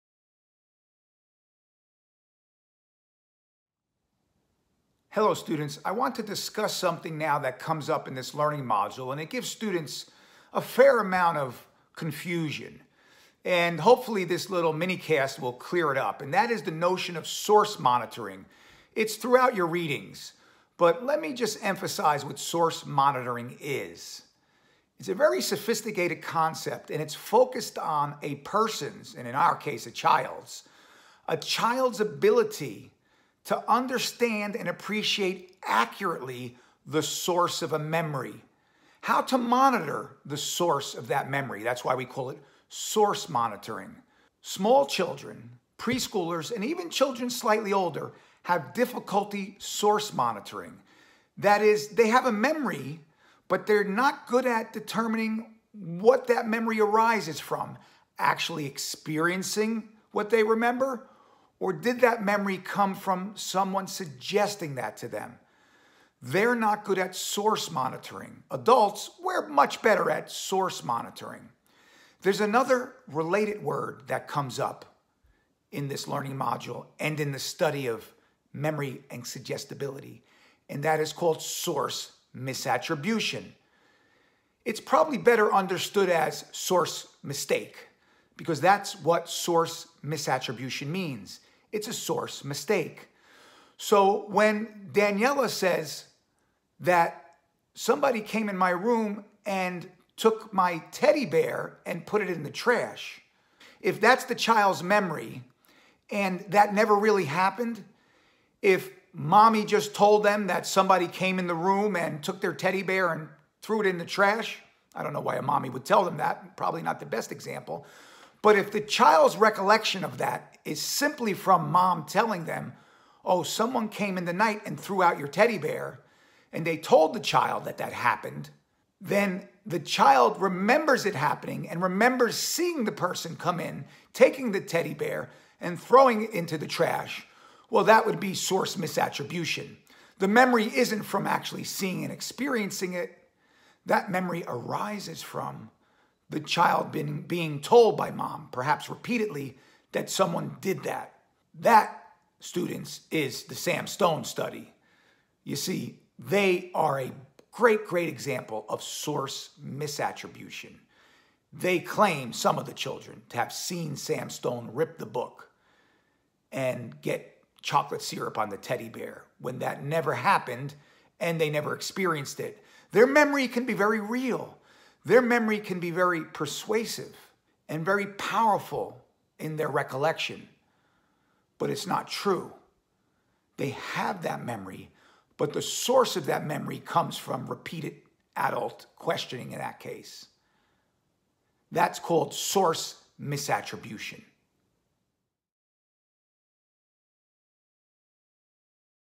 Source Monitoring and Source Misattribution (LM4)
Forensic Interviewing of Children: Professor Del Russo discusses Source Monitoring and Source Misattribution.